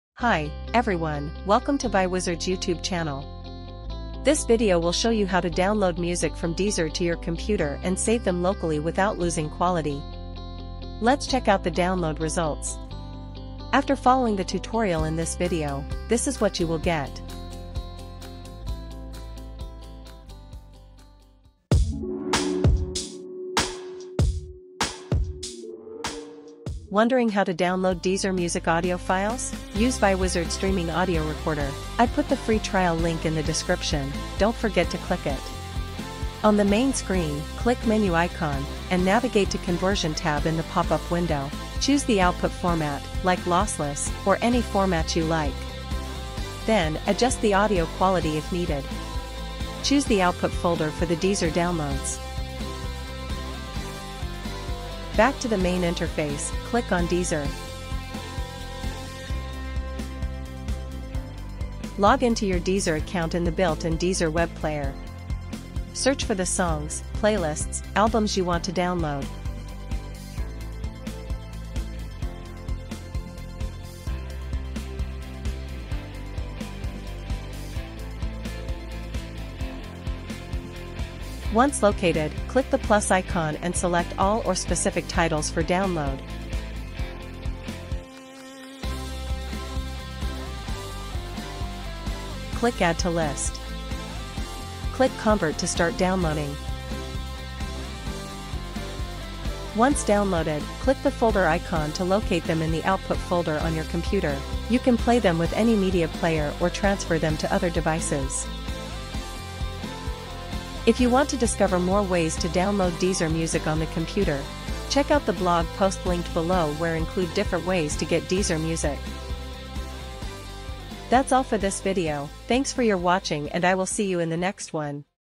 How to Download Music from Deezer to PC [Lossless]
📚 Want more ways to download from Deezer?
🎵 Want to download Deezer music to your computer and keep it forever?
In this video, we’ll show you the easiest way to save Deezer songs in high quality on your PC or Mac!

With ViWizard Streaming Audio Recorder, you can:
✅ Download any Deezer song, playlist, or album
🎶 Keep 100% original sound quality (HiFi supported)
📁 Save music as MP3, FLAC, WAV, AAC, and more
📲 Enjoy offline playback on any device

Contents
0:00 Intro
0:13 What You'll Get
0:27 What You'll Need
1:50 More Ways

🥳 Welcome to the ViWizard YouTube channel! More videos on our Channel are coming to this space.